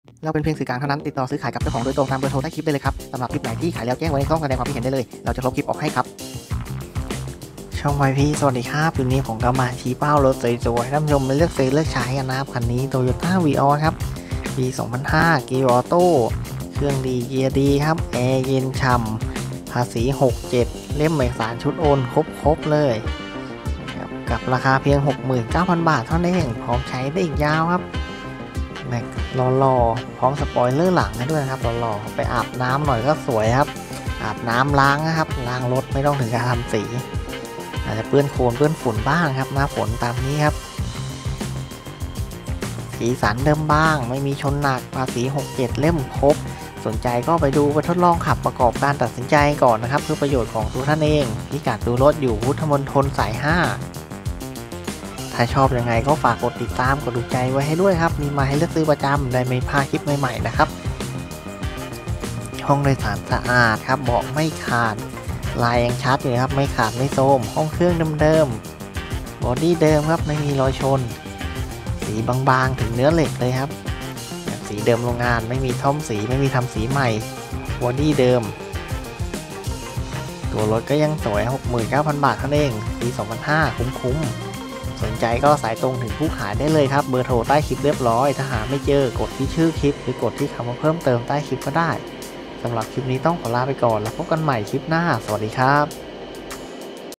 วีออสสวยๆ💥เดิมโรงงาน👍ขายถูก 69,000 บาท
โตโยต้า วีออส ปี2005 เกียร์ออโต้

฿69,000

69,000฿Toyota vios ปี 2005 เกียร์ออโต้
เครื่องเกียร์ดี แอร์เย็น ภาษี67 เล่มพร้อม รถอยู่พุทธมลฑลสาย5 นครปฐม โทร